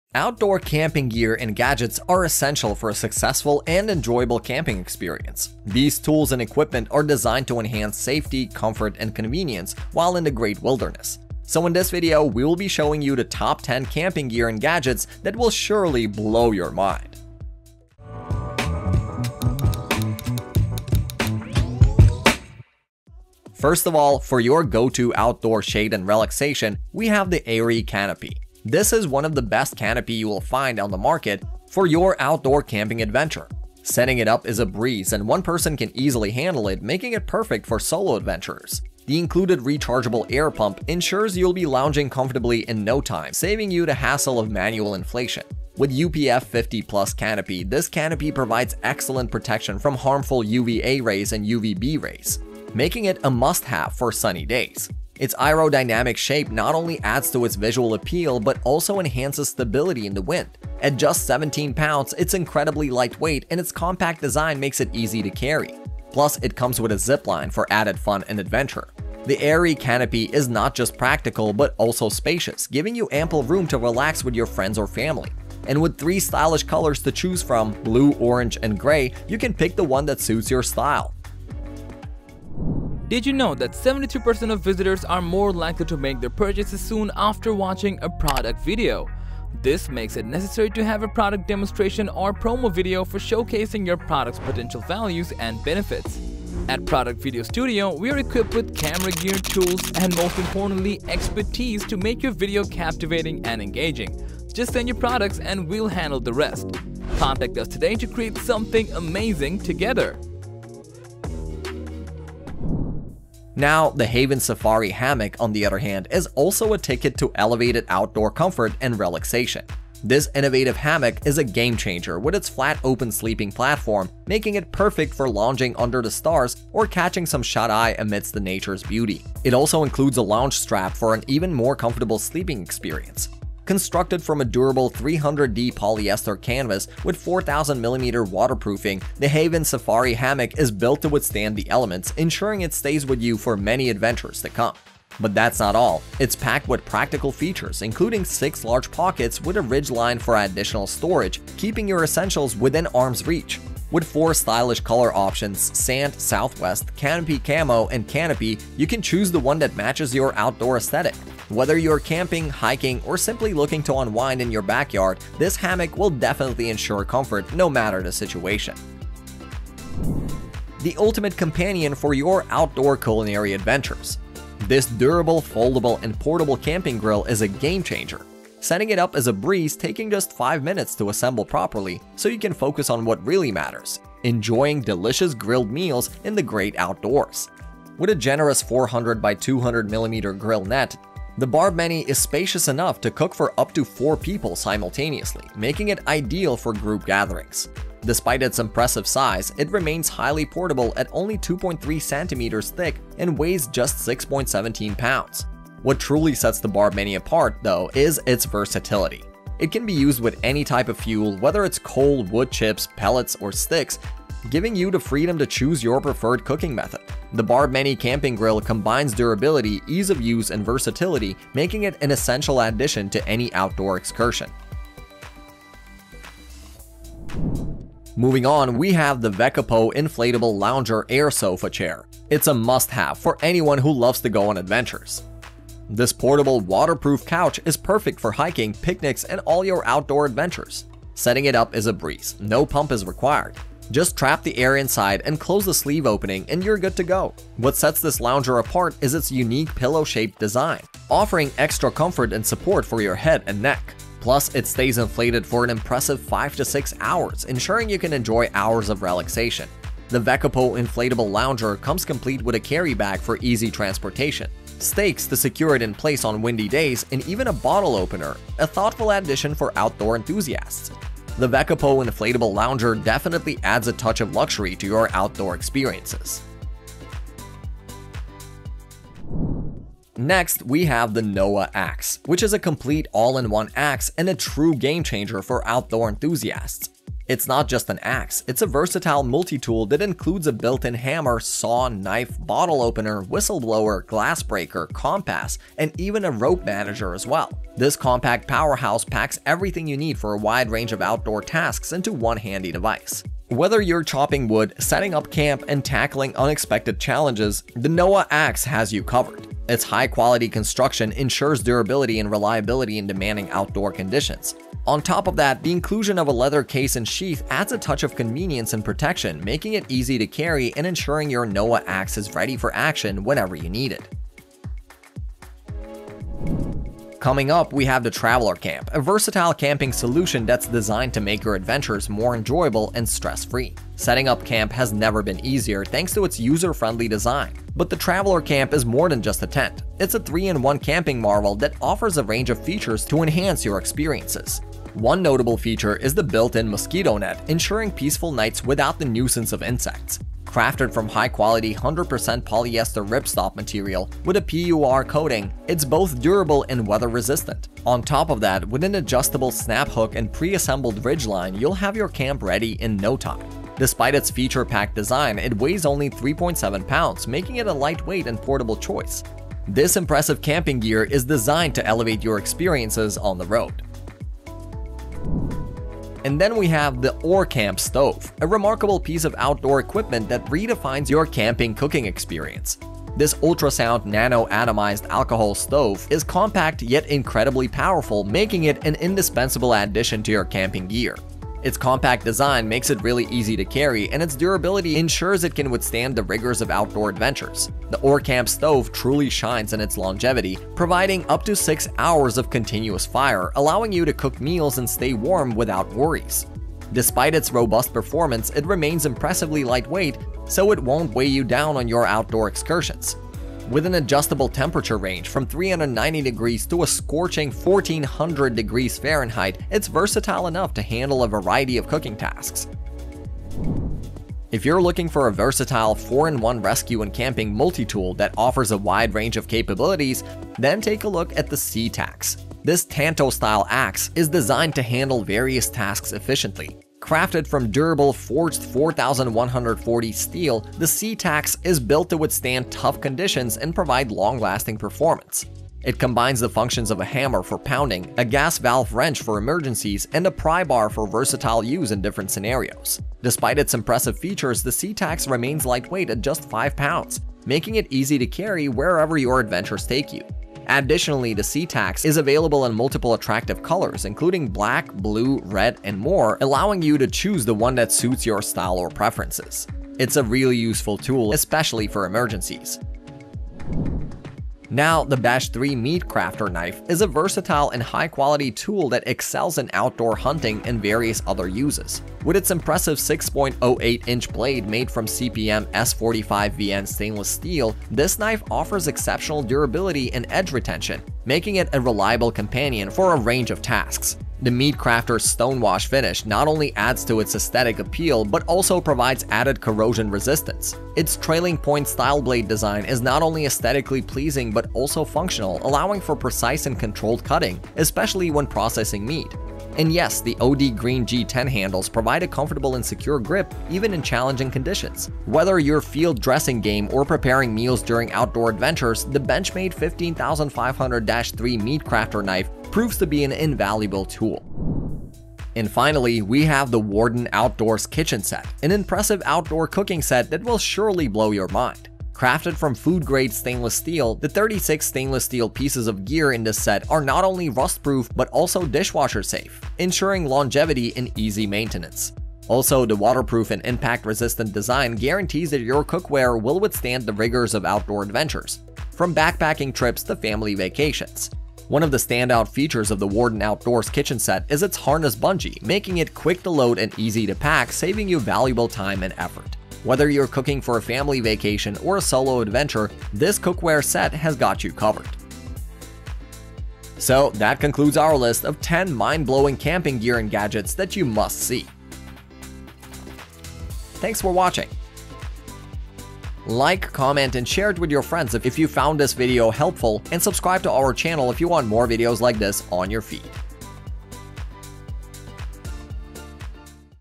10 Mind Blowing Camping Gear & Gadgets You Must See!
Camping gear and gadgets are essential companions for any outdoor enthusiast undertaking on a thrilling hiking or wilderness adventure. These tools not only enhance the experience but can also prove to be invaluable lifesavers. Here, we present the top 10 recommendations for camping gear and gadgets that are sure to captivate your imagination and elevate your outdoor adventures.

00:00 - Introduction

00:22 - 10. Aeri

01:52 - 9. Haven Safari

02:50 - 8. Barbemany

03:47 - 7. WEKAPO Inflatable Lounger

04:39 - 6. NOA

05:26 - 5. Traveller Camp

06:15 - 4. OrCamp Stove

07:03 - 3. The CTAX

07:53 - 2. Benchmade 15500-3 Meatcrafter

08:44 - 1. Warden Outdoors Kitchen Set